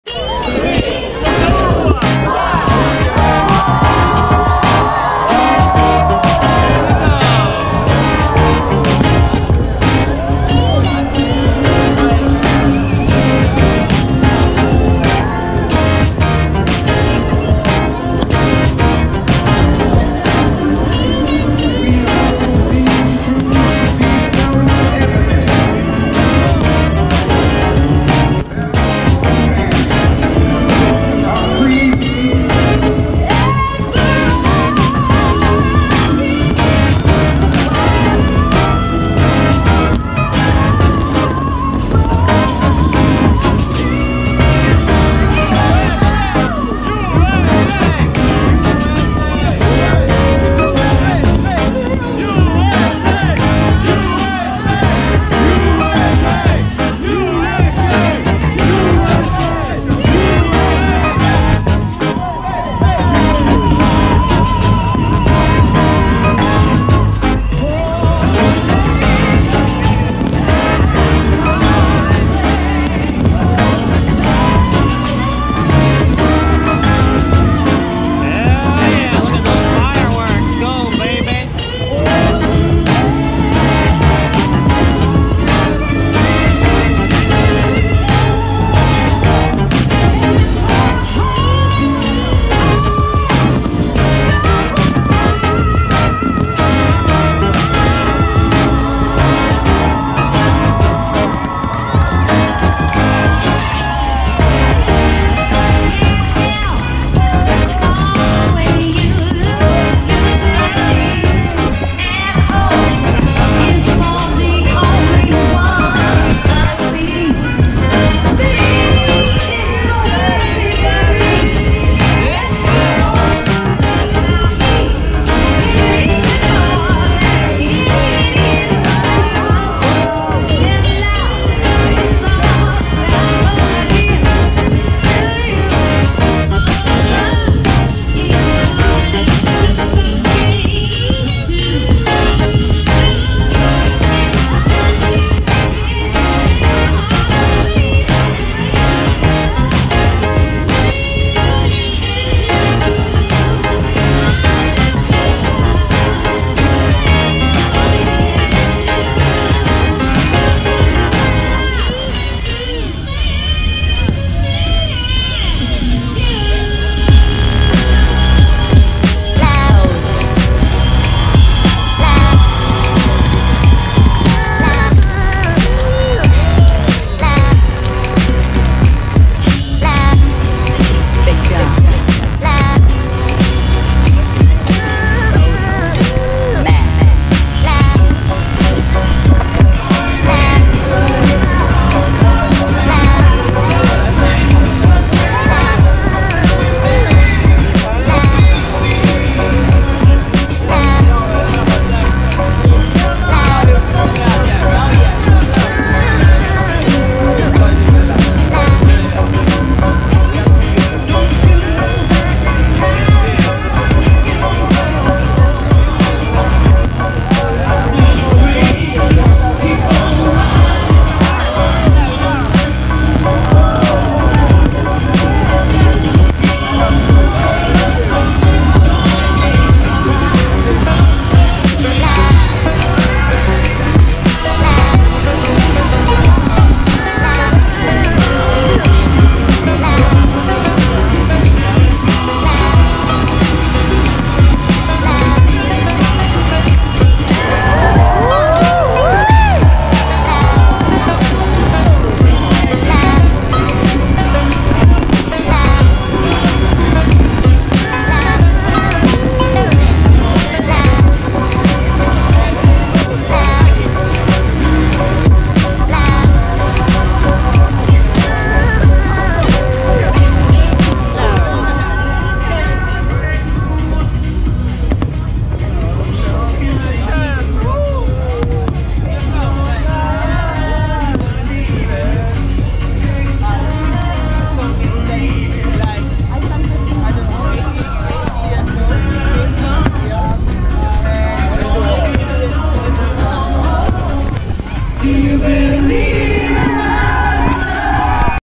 Philadelphia, PA independence day 2018
It was last year when me, Brandon, and Lance went to downtown Philadelphia to see not only the fireworks, but the party and the live performance by Mr 305 Pitbull. What a difference a year make.